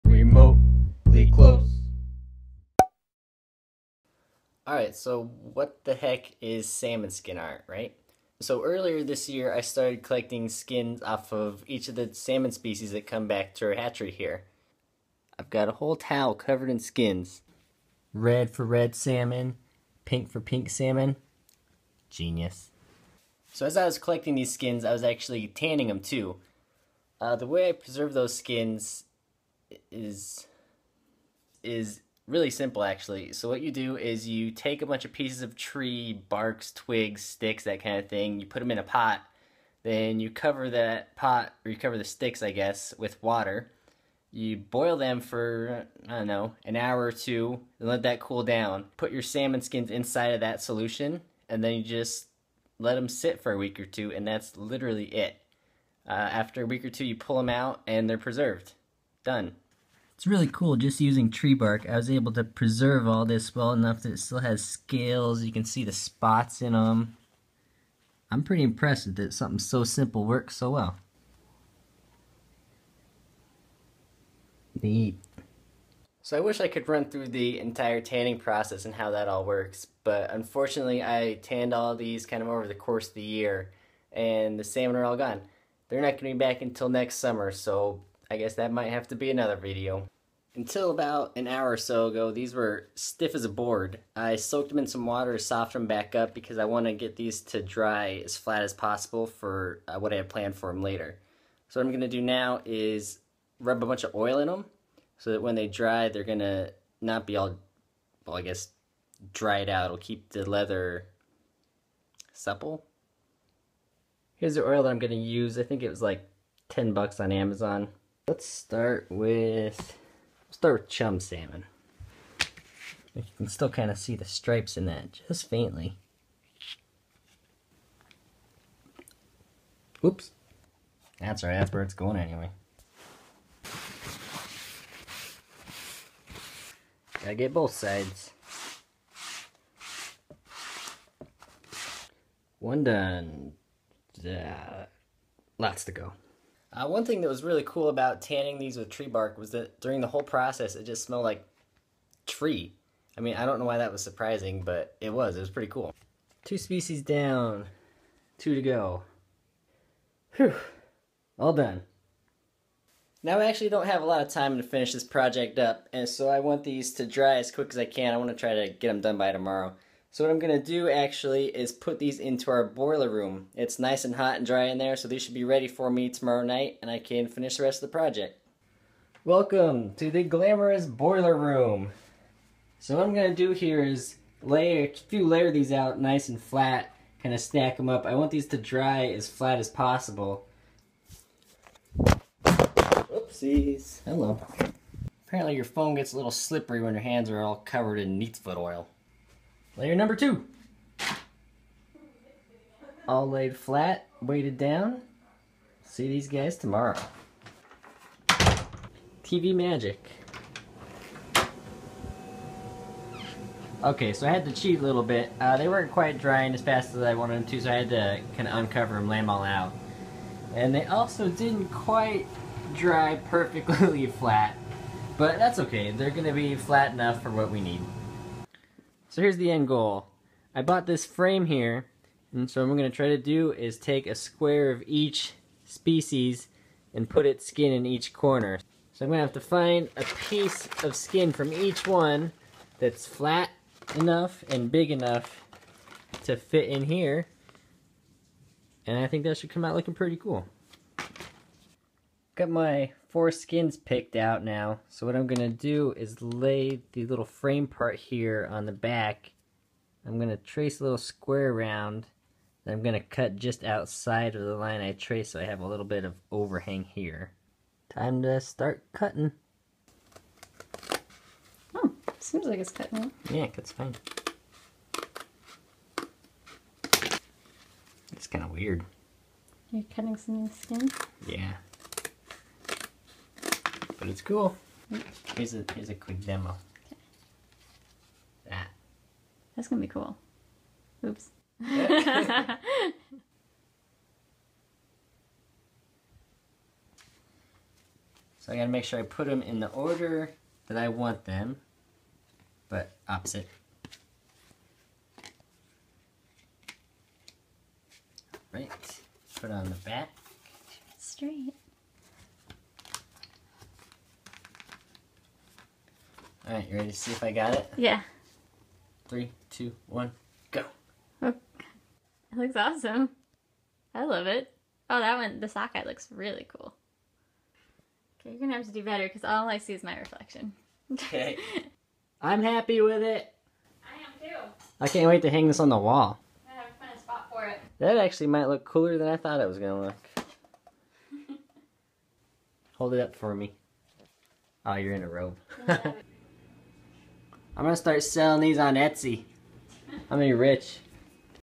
Turning Salmon Leather in to Art
All you need to turn fish skin in to leather is... tree bark?